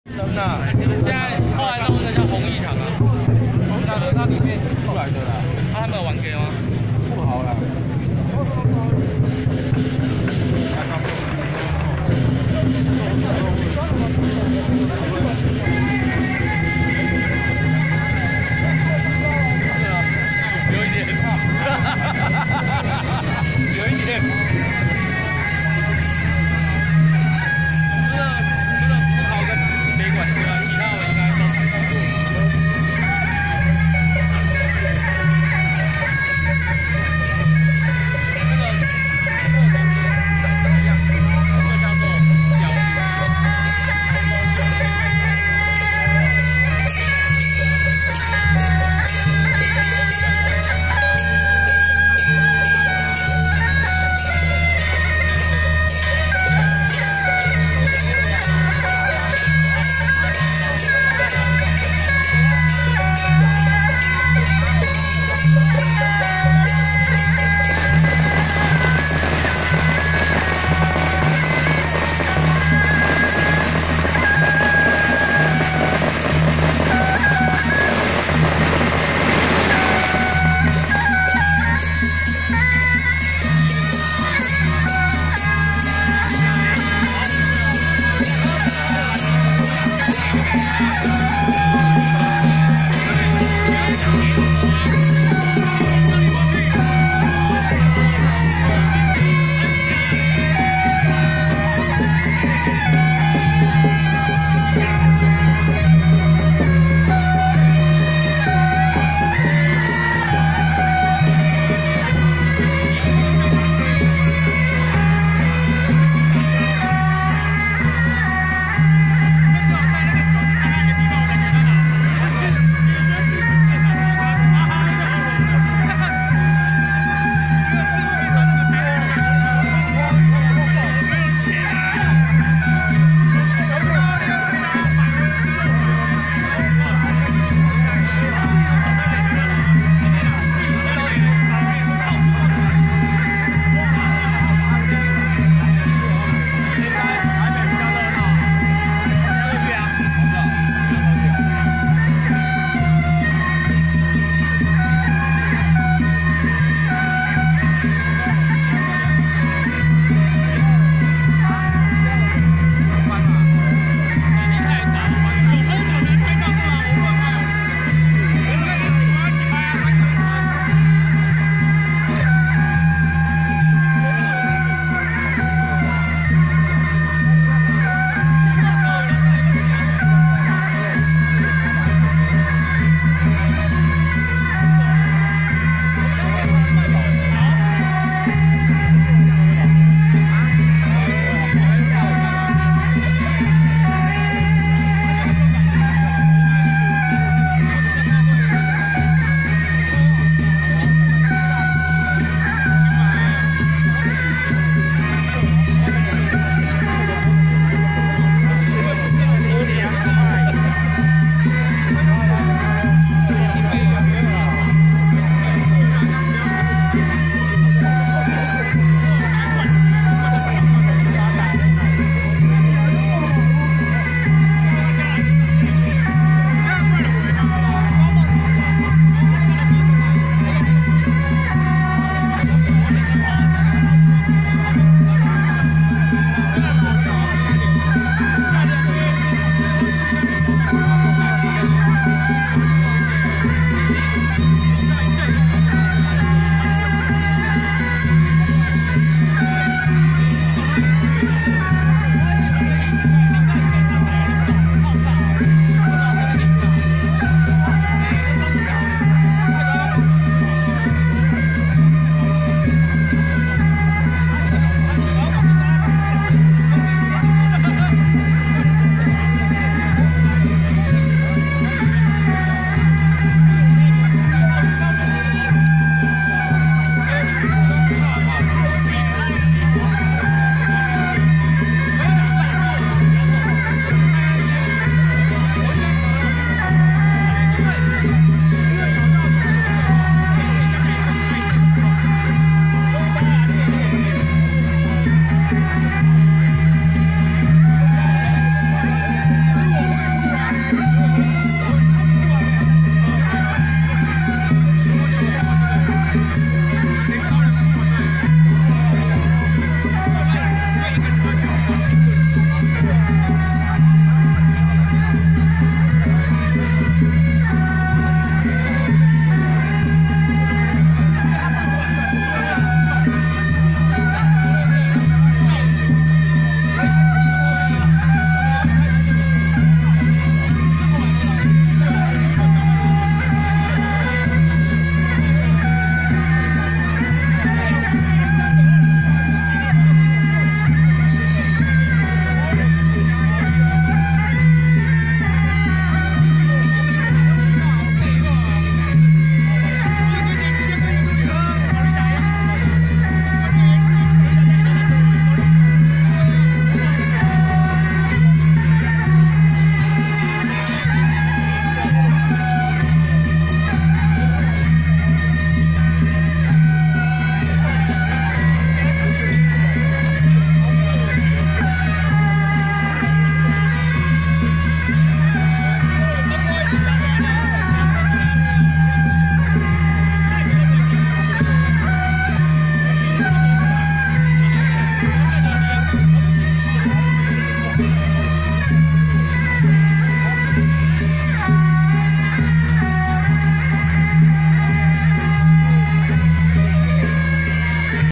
108木柵集應廟九年輪祀慶典恭祝保儀尊王聖誕遶境【景美會元洞-神將團】
景美會元洞-神將團